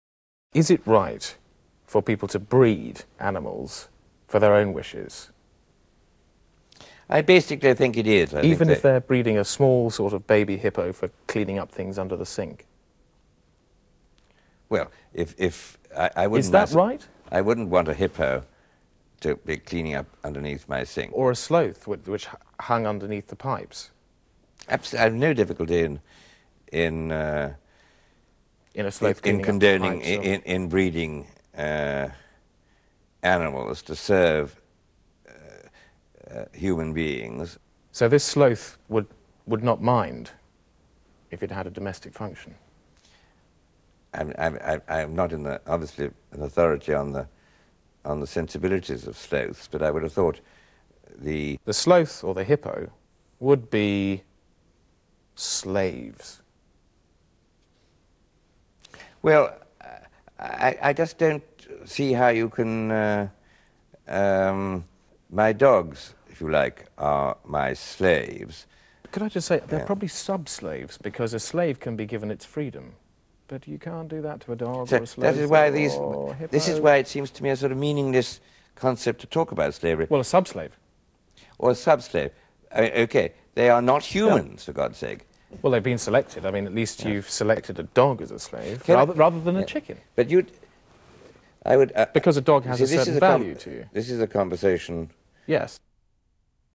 David Jatt interviews famed journalist Sir Peregrine Worsthorne
Are we right to keep and breed animals?